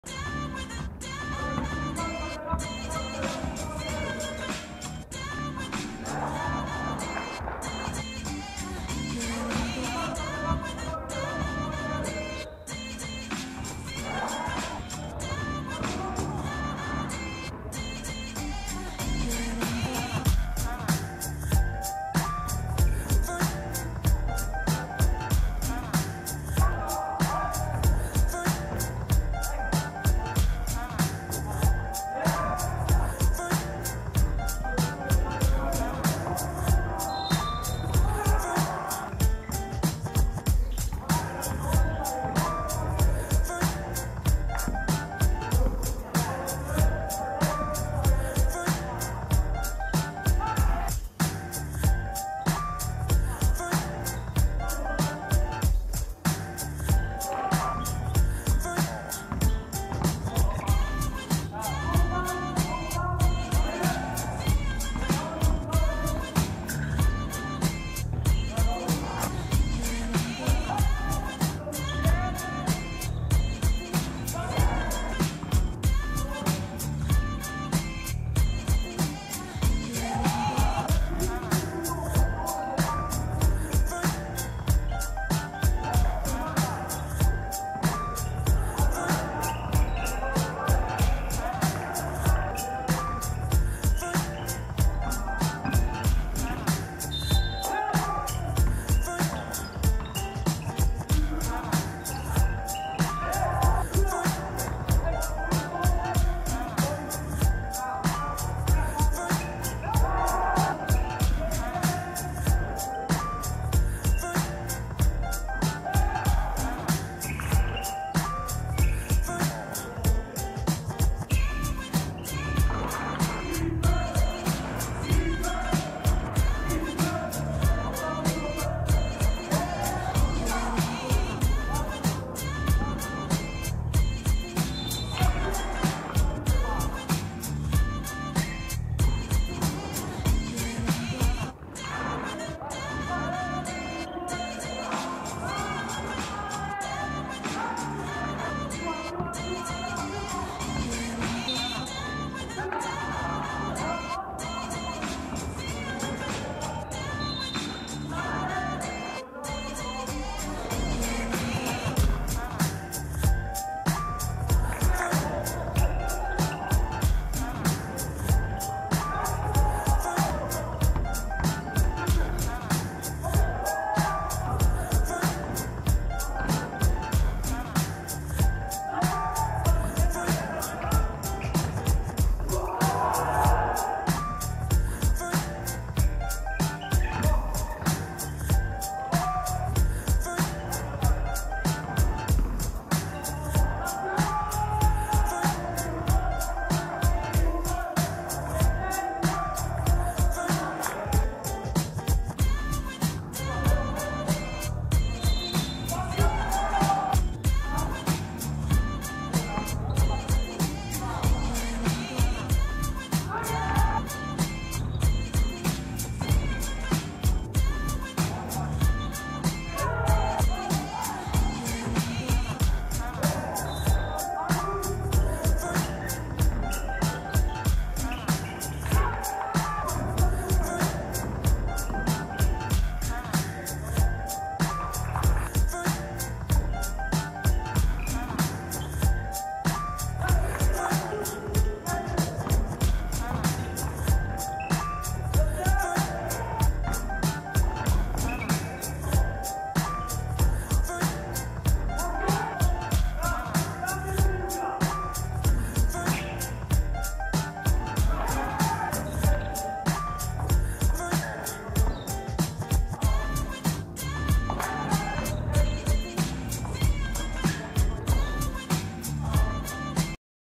10 07 Boston vs Dallas – Draft Camp 26 Brodnica
BOSTON 75:94 DALLAS
M.Prałat 29pkt 11-13fg 6zb 5ast
T.Fic 16pkt 13zb 9ast